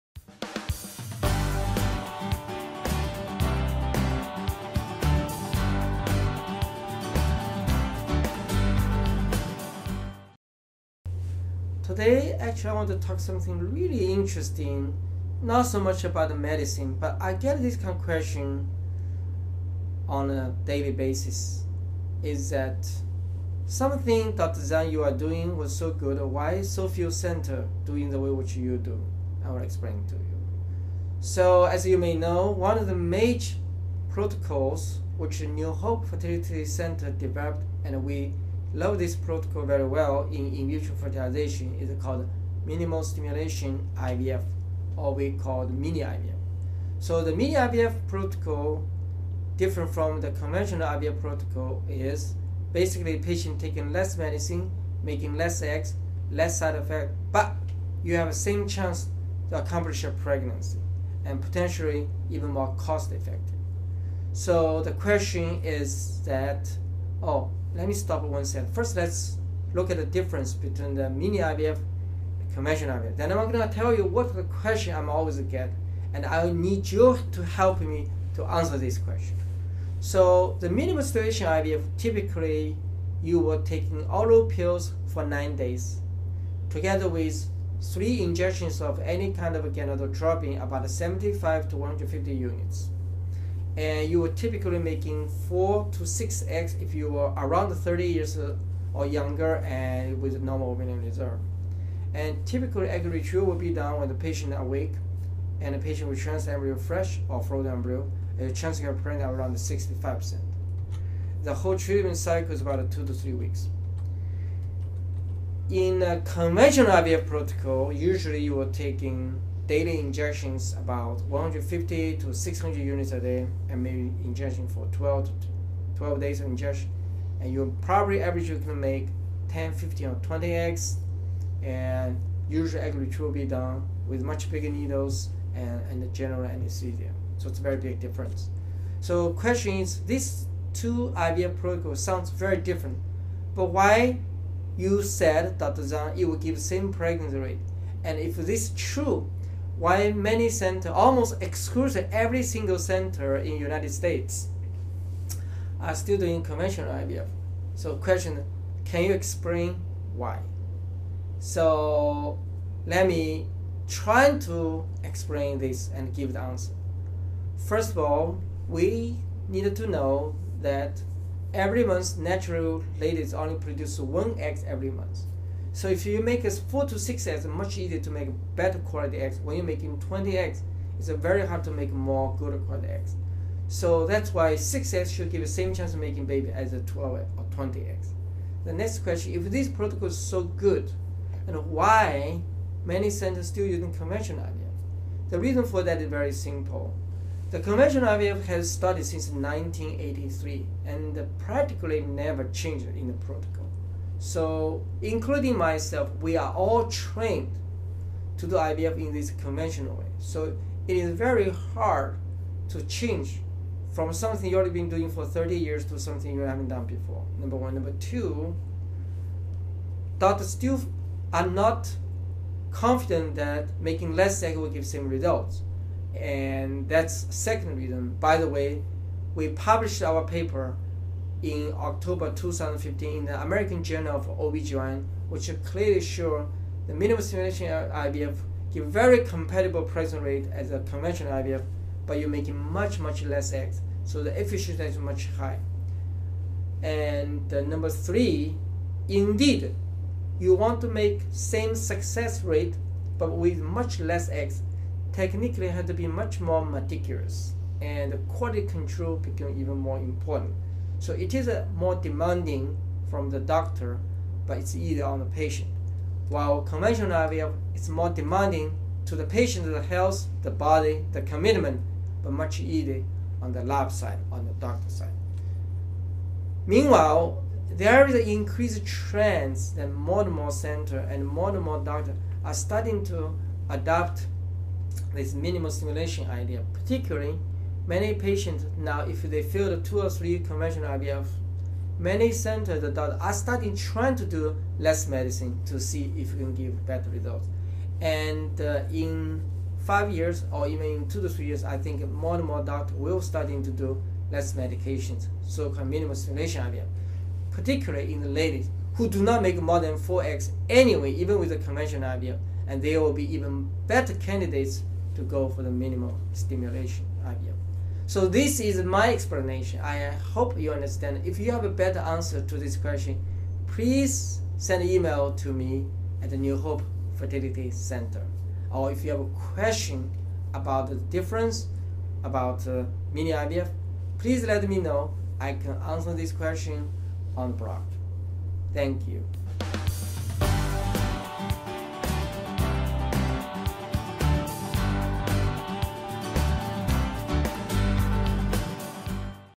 Why isn't Mini IVF used more widely? New Hope Fertility Center NYC
Today, I wanted to talk about something really interesting, not so much about medicine. But I get this kind of question on a daily basis is that something that Dr. Zhang you are doing is so good but why does so few centers do what you do. I will explain it to you.

So, as you may know, one of the major protocols which New Hope Fertility Center developed, and we love this protocol very well in in-vitro fertilization, is called minimal stimulation IVF, or we call Mini-IVF. The Mini-IVF protocol different from the conventional IVF protocol is basically patient taking less medicine, making less eggs, less side effects, but you have the same chance to accomplish pregnancy and potentially even most cost-effective.

But first look at the difference between the Mini-IVF and Conventional IVF. Then I am going to tell you what is the question I always get and I will need YOU to help me to answer this question.
Minimal stimulation IVF typically, you will take an oral pill for 9 days, together with 3 injections of any kind of gonadotropin, about 75 to 150 units. You would typically make 4 to 6 eggs if you are around 30 years or younger and with normal ovarian reserve. Typically, egg retrieval will be done with the patient awake and the patient will transfer fresh or frozen embryo, will have a chance of pregnant around 65%. The whole treatment cycle is about 2 to 3 weeks.

In conventional IVF protocol, usually you would take daily injections about 150 to 600 units a day and maybe injection for 12 days. You probably make an average of 10, 15, or 20 eggs and usually egg retrieval with much bigger needles and general anesthesia. So there’s a very big difference.

Question is, these two IVF protocols sounds very different but why did you say, Dr. Zhang, that it will give the same pregnancy rates. And if this is true, why many center, almost all exclusive, every single centers in US are still doing conventional IVF? Question, can you explain why? Let me try to explain this and give an answer.

First of all, we needed to know that every month, naturally, ladies only produce one egg every month. So if you make 4 to 6 eggs, it is much easier to make better quality eggs. When you making 20 eggs, it is very hard to make more good quality eggs. So that is why 6 eggs should give the same chance of making babies as compared to 12 or 20 eggs.

The next question is, if this protocol is so good, why many centers still uses conventional IVF? The reason for that is very simple. The conventional IVF has started since 1983, and it practically never changed in protocol. So, including myself, we are all trained to do IVF in this conventional way. It is very hard to change from something that you have been doing for 30 years to something you have never done before, number 1.

Number 2, doctors are still not confident that making less eggs will give the same results. That’s the second reason. By the way, we published our paper in October 2015 in American Journal of OBGYN, which clearly shows that minimal stimulation IVF give very comparable as conventional IVF, but you make much less eggs, so the efficiency is much higher.

Number 3, indeed, you want to make same success rates but with much less egg, techniques has to be much more meticulous. Quality control becomes even more important. It is a more demanding from the doctors, but easier on the patients. While conventional IVF is more demanding to the patient’s health and the body, the commitment, but much easier on the lab side, and the doctor’s side.

Meanwhile, there is an increase trend that more and more centers and more and more doctors are starting to adopt this minimal stimulation idea. Particularly, many patients now if they failed 2 or 3 conventional IVF, many center’s doctors are starting to try to do less medicine to see if it can give better results. In 5 year, or even in 2 to 3 years, I think more and more doctors will start to do less medication, so called minimal stimulation IVF.  Particularly, ladies who do not make more than 4 eggs anyway, even with conventional IVF and they will be even better candidate to go for minimal stimulation IVF.

So this is my explanation. I hope you understand. If you have a better answer to this question, please send an email to me at New Hope Fertility Center. Or if you have questions about the difference, about Mini-IVF, please let me know, I can answer this questions on my blogs.